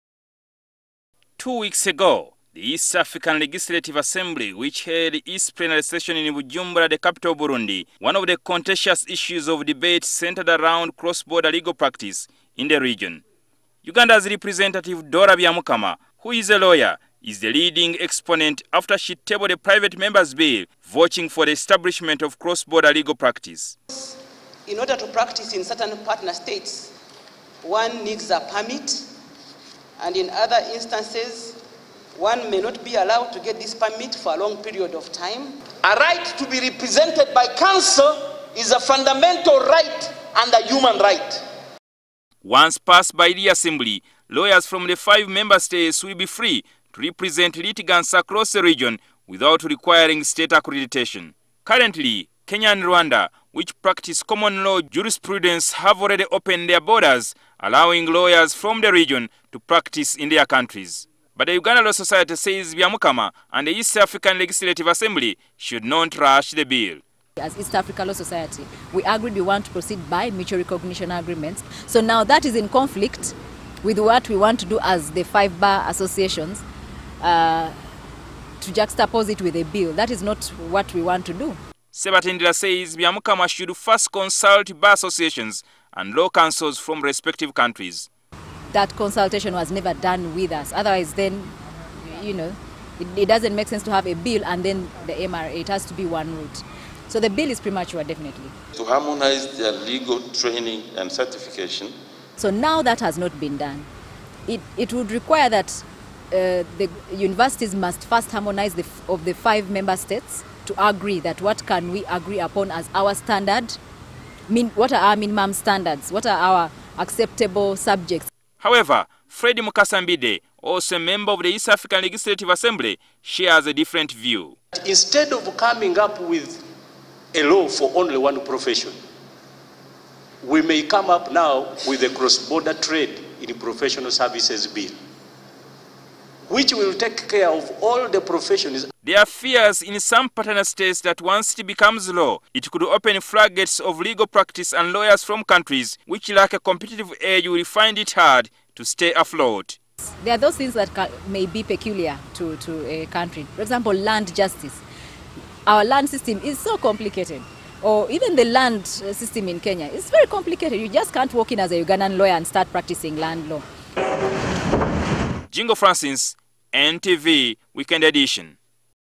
Ugandan lawyers oppose law allowing cross-border practice
The Uganda  Law society is rejecting what they say is a rushed move by the East African Legislative Assembly to enact  a law that will allow cross border legal practice in the region.

The President of Uganda law society, Ruth Ssebatindira, says Ugandan lawyers prefer mutual recognition agreements signed as opposed to the private members bill moved by Dora Byamukama in the regional assembly vouching for the implementation of cross-border legal practice.